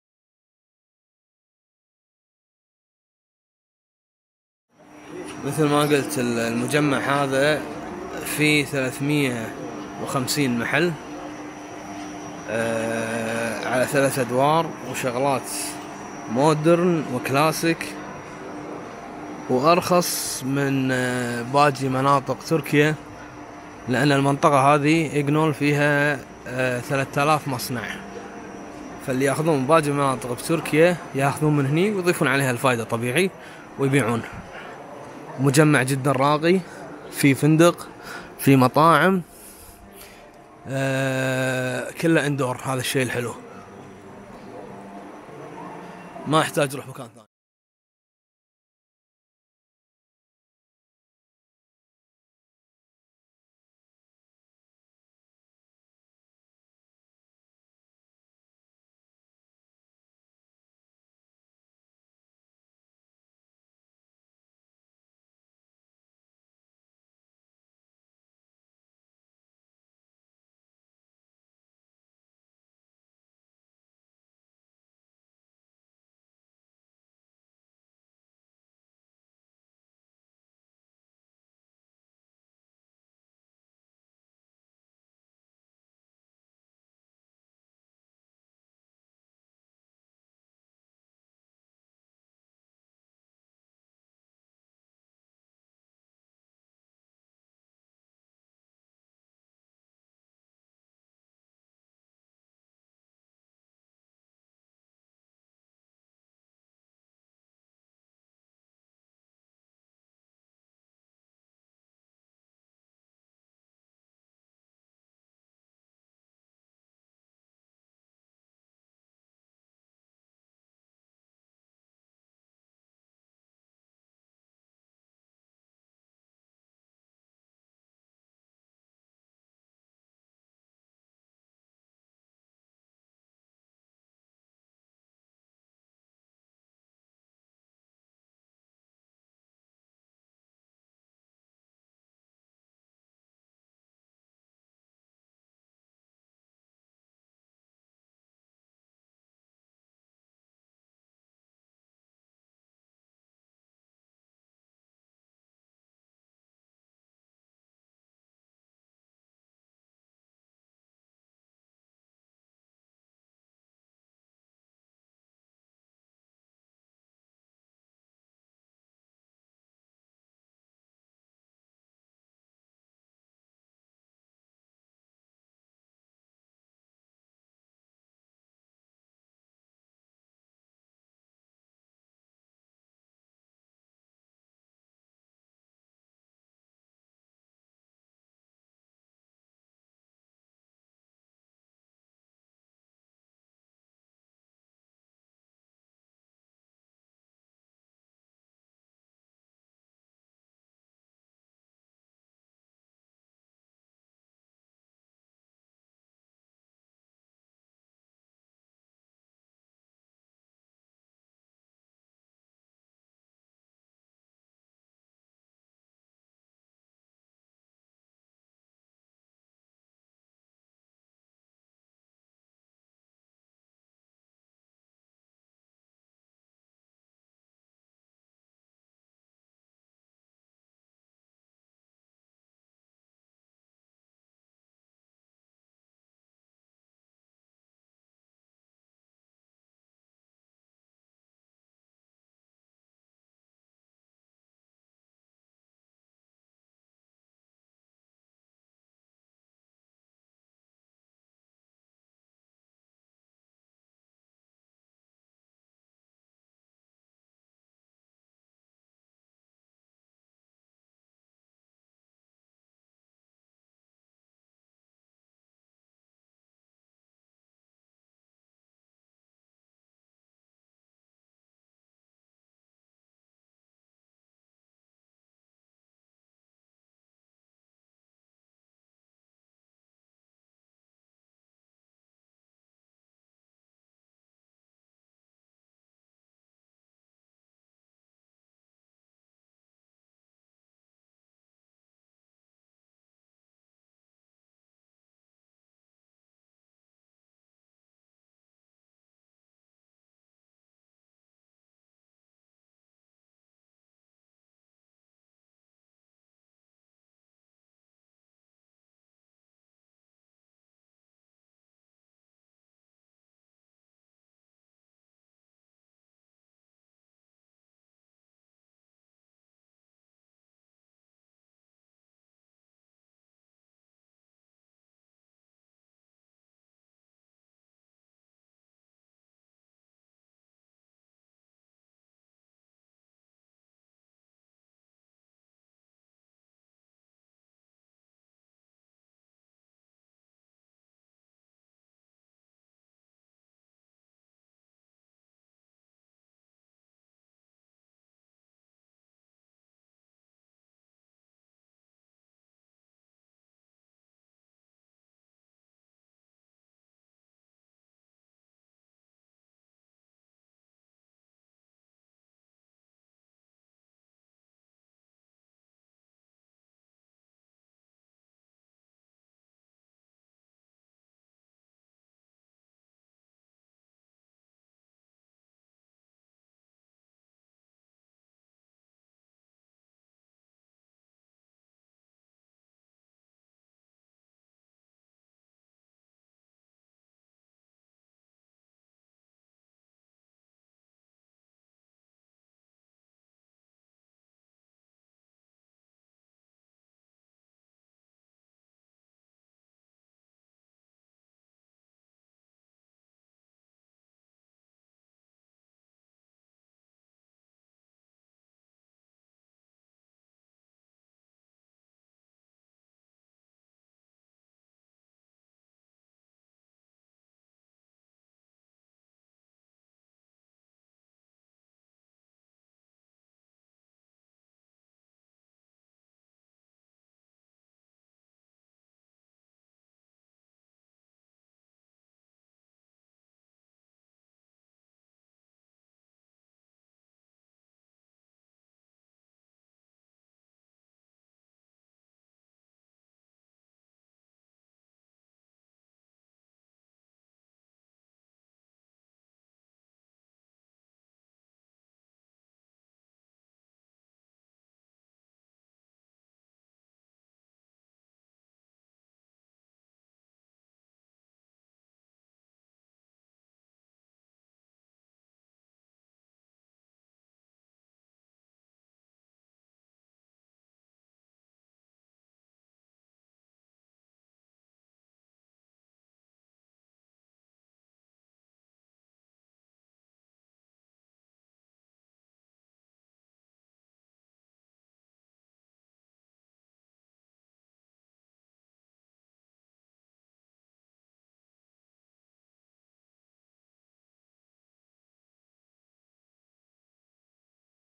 الاثاث التركي
رحلة الي مجمعات الاثاث في تركيا